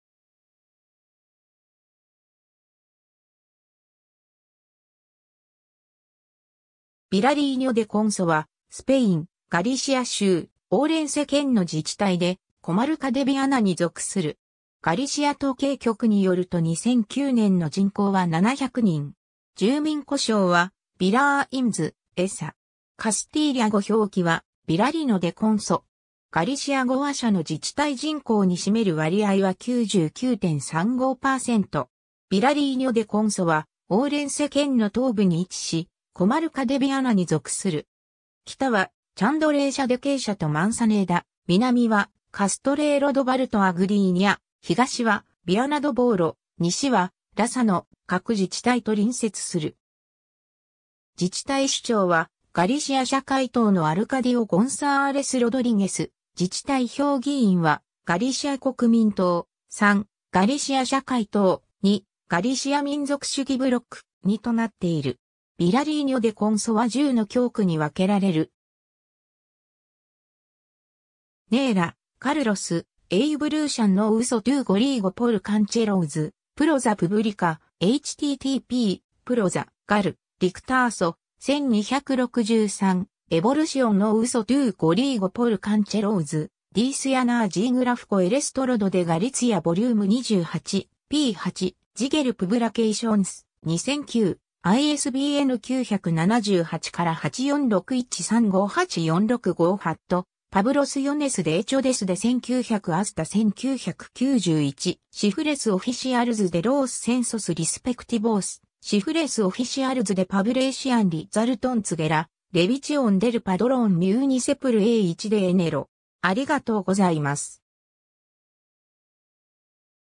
ビラリーニョ・デ・コンソ
ビラリーニョ・デ・コンソ（Vilariño de Conso）はスペイン、ガリシア州、オウレンセ県の自治体で、コマルカ・デ・ビアナに属する。
ガリシア統計局によると2009年の人口は700人（2003年：771人）。
住民呼称はvilariñés/-esa。
カスティーリャ語表記はVillarino de Conso （ビジャリーノ・デ・コンソ）。
 ビラリーニョ・デ・コンソはオウレンセ県の東部に位置し、コマルカ・デ・ビアナに属する。
北はチャンドレーシャ・デ・ケイシャとマンサネーダ、南はカストレーロ・ド・バルとア・グディーニャ、東はビアナ・ド・ボーロ、西はラサの各自治体と隣接する。
 自治体首長はガリシア社会党（PSdeG-PSOE）のアルカディオ・ゴンサーレス・ロドリゲス（Arcadio González Rodríguez）、自治体評議員はガリシア国民党（PPdeG）：3、ガリシア社会党：2、ガリシア民族主義ブロック（BNG）：2となっている（2007年の自治体選挙の結果、得票順）。
 ビラリーニョ・デ・コンソは10の教区に分けられる。
^ Neira, Carlos (ガリシア語), Evolución no uso do galego por concellos, Praza Pública., ^ Dicionario Xeográfico Ilustrado de Galicia vol.28、p.8、Xeogal Publicacións、2009、ISBN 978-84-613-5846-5 ^ Poblaciones de hecho desde 1900 hasta 1991. Cifras oficiales de los Censos respectivos. ^ Cifras oficiales de población resultantes de la revisión del Padrón municipal a 1 de enero.